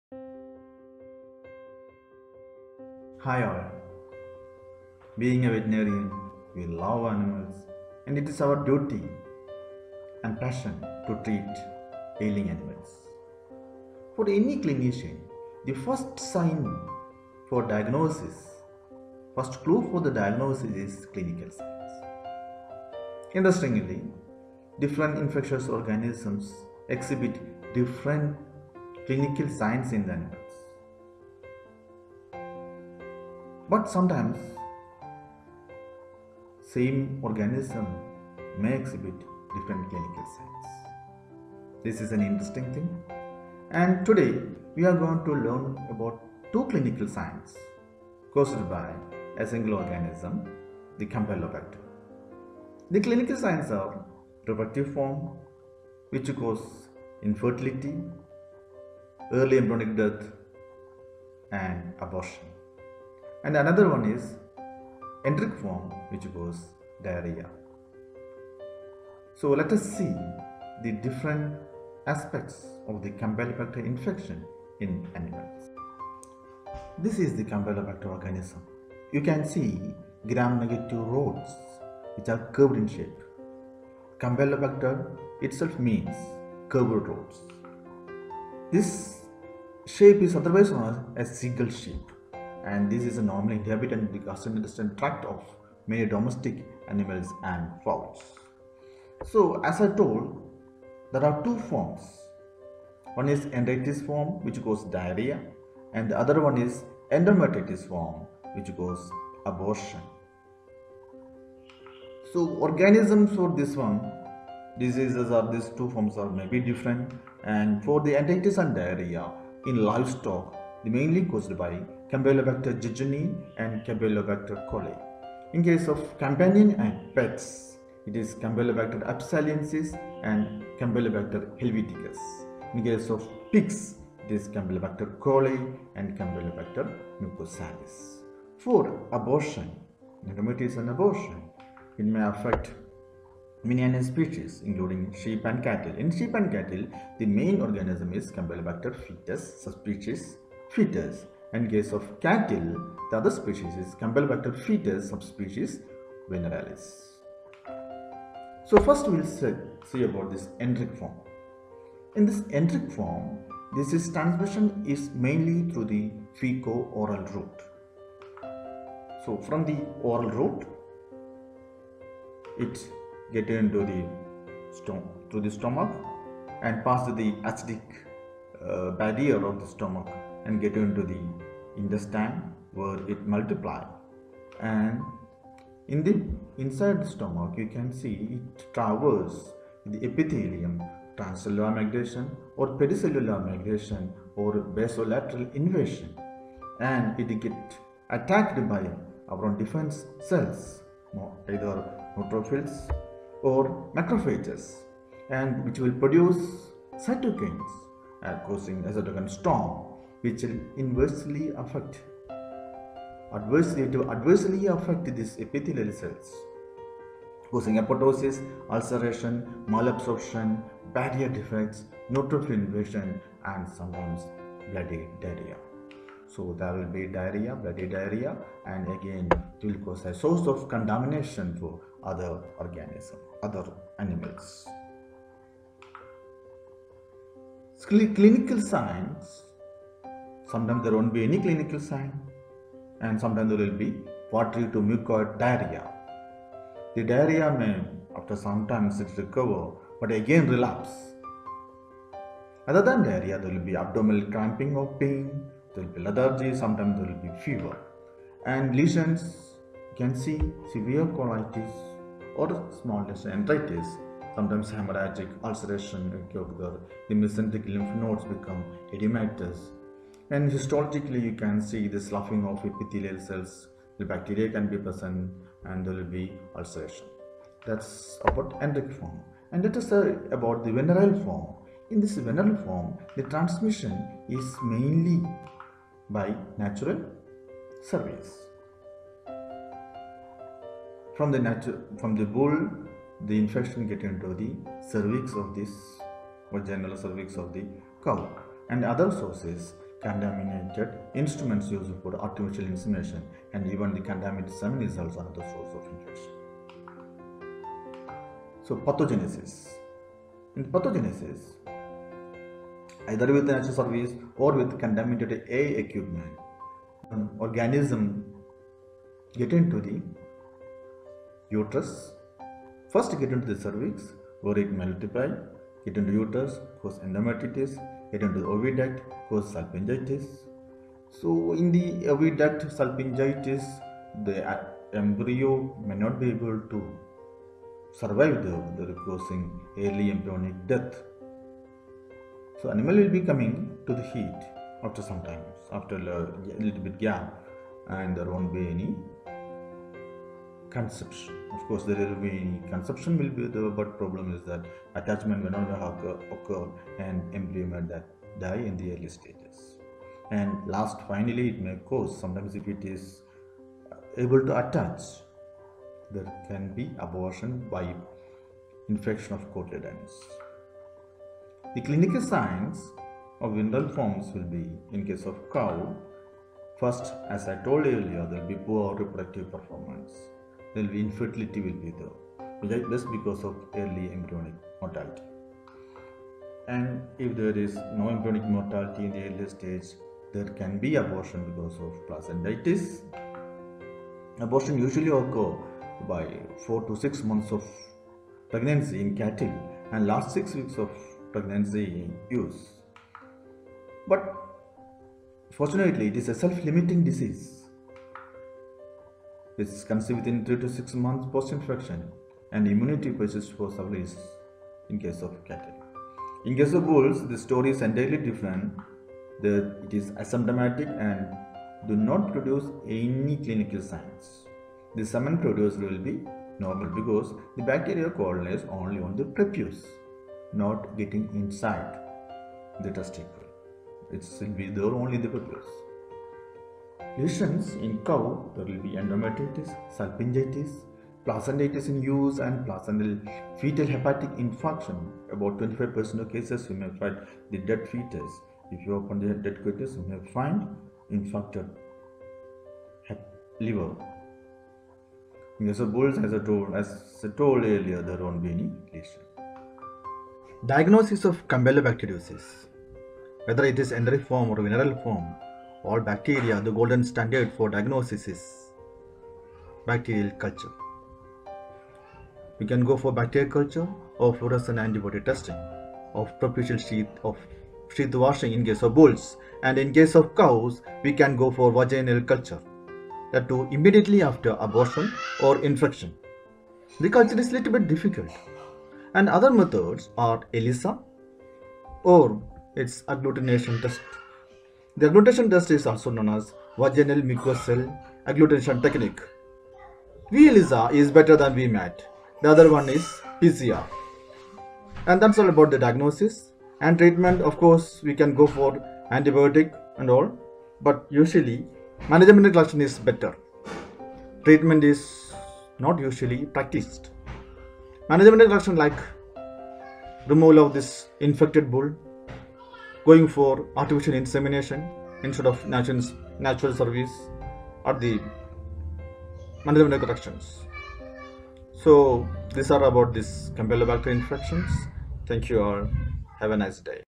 Campylobacter infection in animals
This video is to help Veterinary students to have basic knowledge in Veterinary Pathology. This is only for academic purpose.